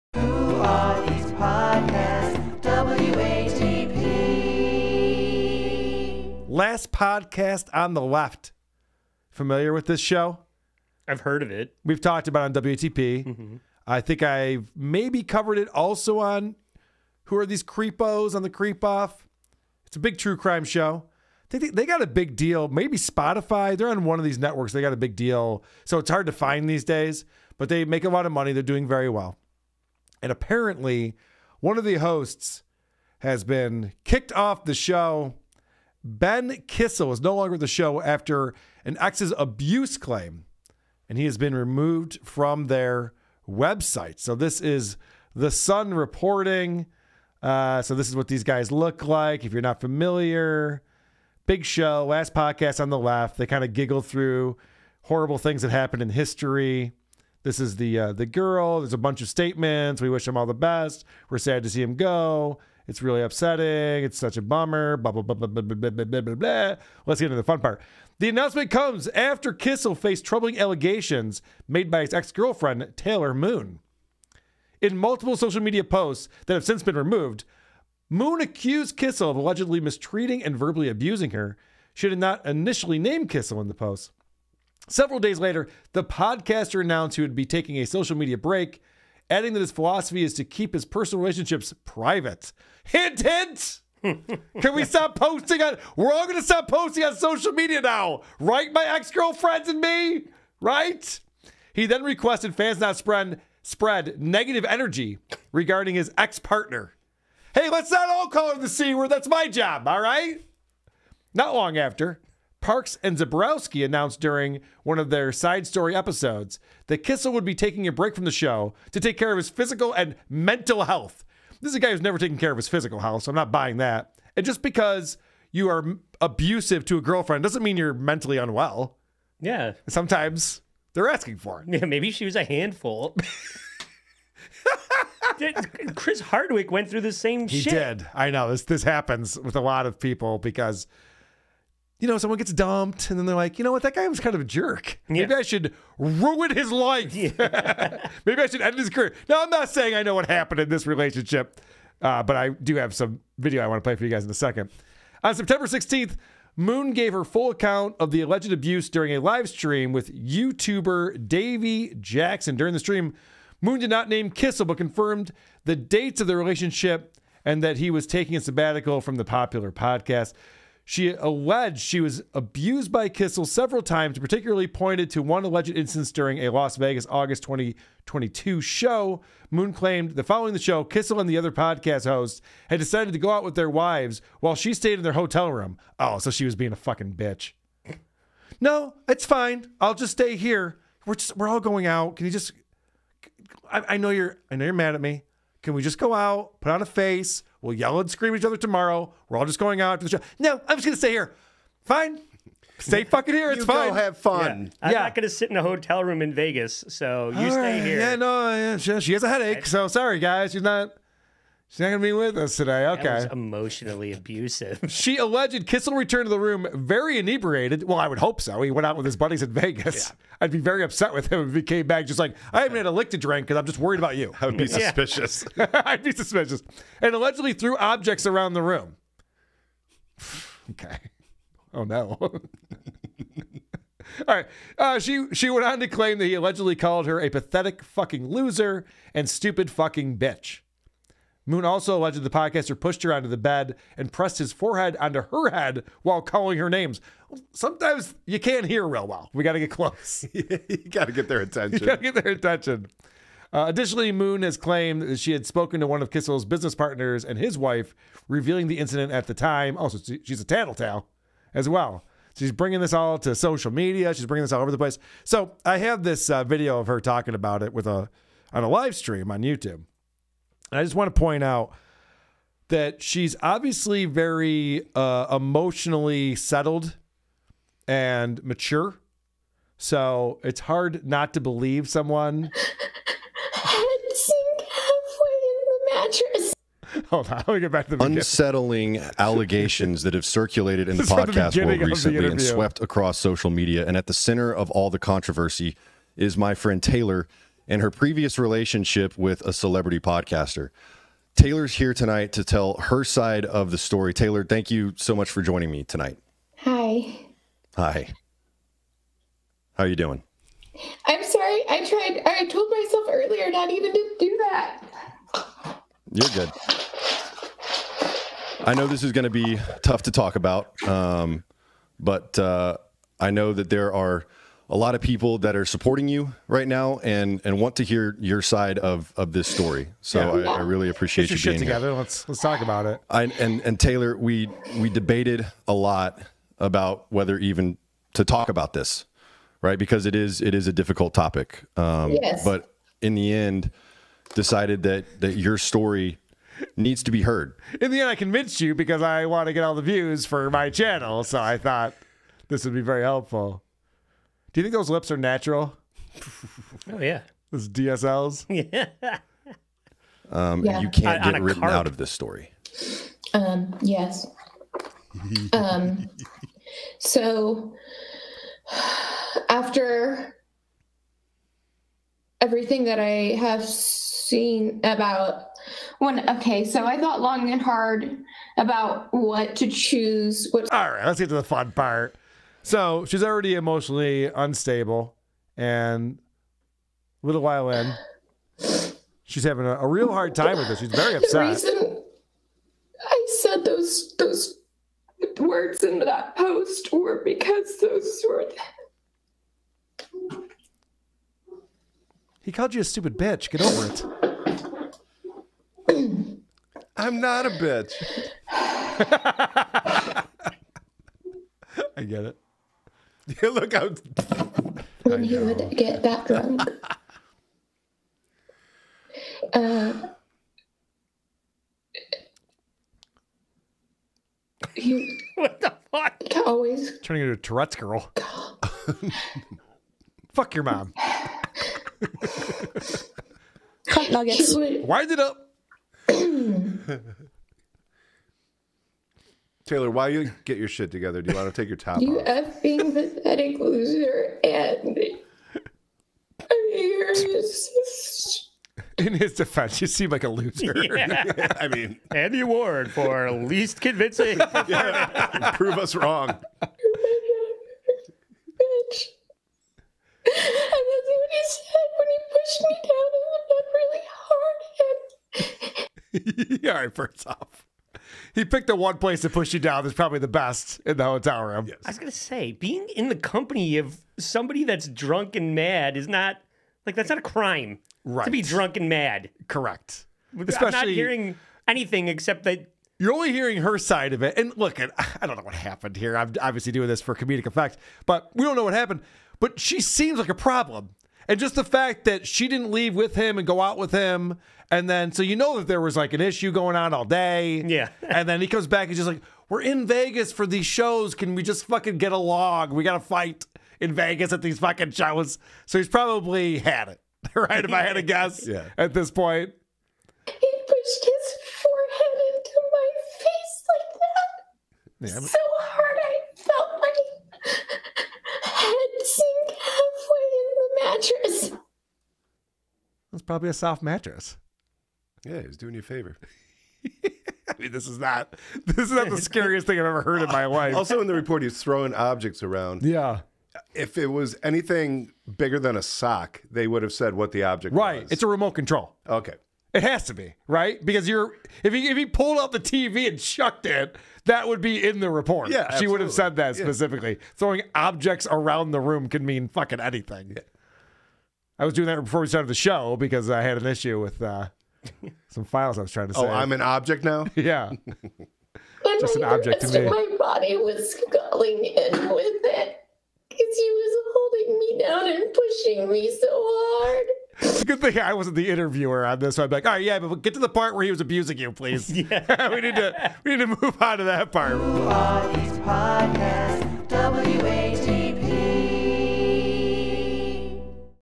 Last Podcast on the Left Host Gets BLOWN OUT Based Only On Allegations From An Ex!
I'm not a big fan of Last Podcast on the Left but what I hate more is the idea that ex-girlfriends can ruin their ex's lives by telling one-sided stories. It happened to Justin Roiland (although that hurt us more than it hurt him) and we see it all over from Bryan Callen to poor Harvey Weinstein. It seems Ben Kissel is the latest victim.

Watch the unedited livestream every Saturday and Wednesday and get bonus episodes by signing up or by becoming a member on YouTube.

This is an excerpt from "Who Are These Podcasts?" episode 452 - "John Boy and Billy Big Show"